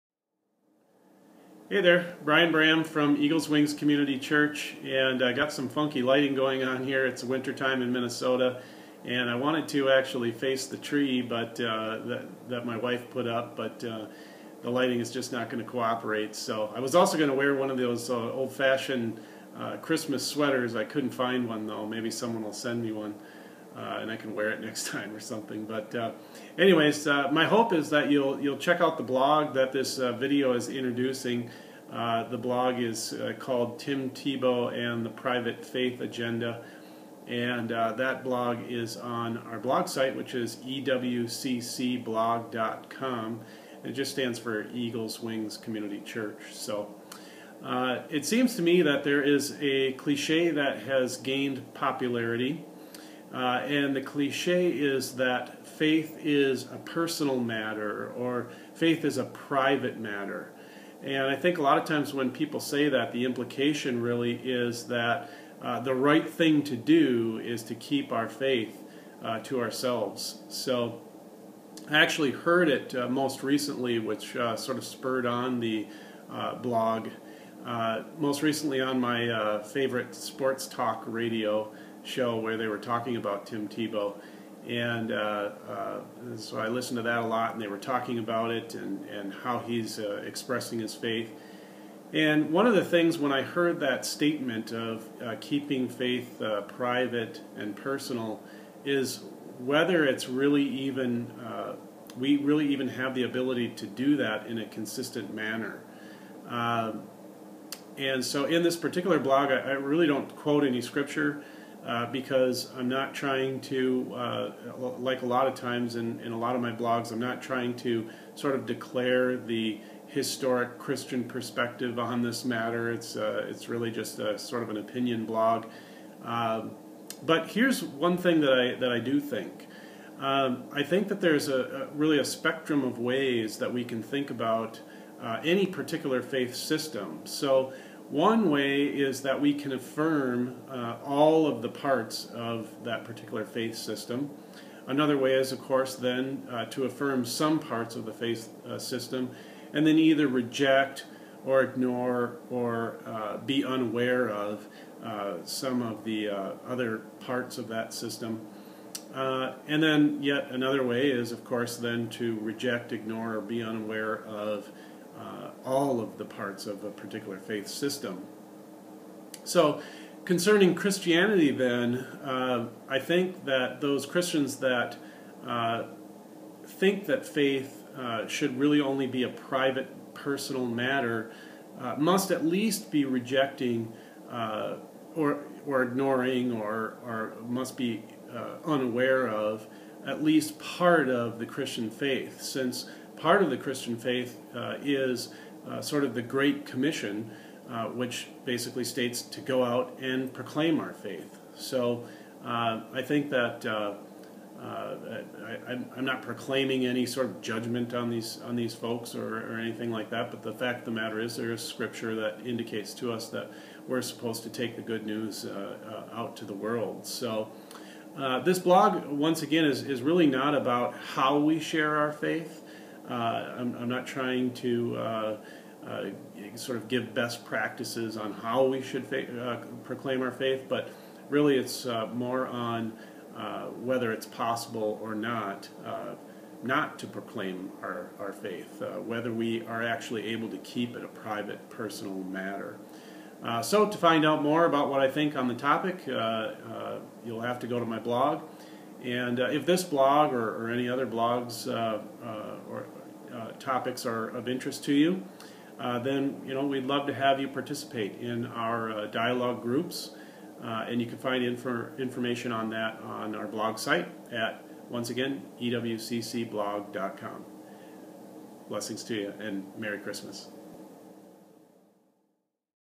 Tim Tebow and the Private Faith Agenda - ewccblog.com - Eagle's Wings Community Church
Can faith ever be only a personal private matter?